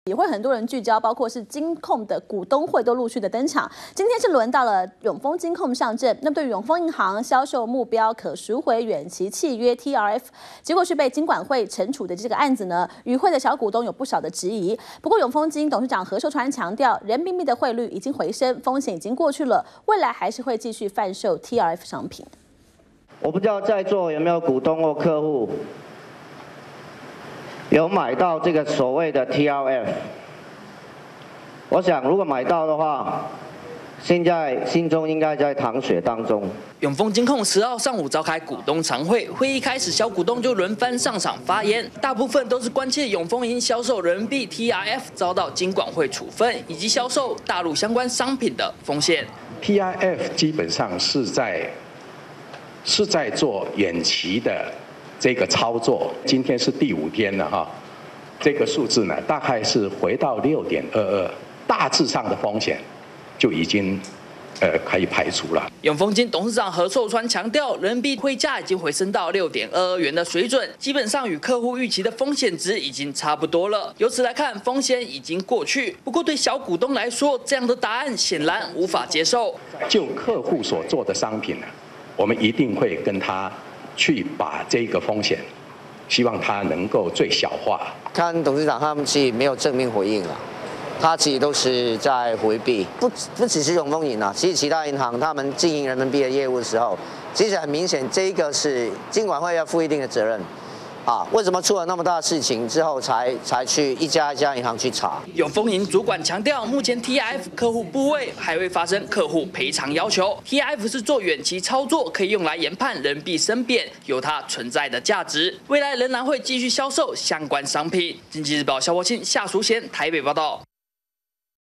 【2014.06.12】小股東質疑TRF 永豐金：風險已控制 -udn tv
金控股東會輪番登場，今天輪到永豐金控上陣，對於永豐銀行銷售目標可贖回遠期契約TRF，結果被金管會懲處一事，與會小股東有不少質疑。不過，永豐金董事長何壽川強調，人民幣匯率已經回升，風險已經過去，未來還是會繼續販賣TRF商品。完整內容 , FB , Twitter , G+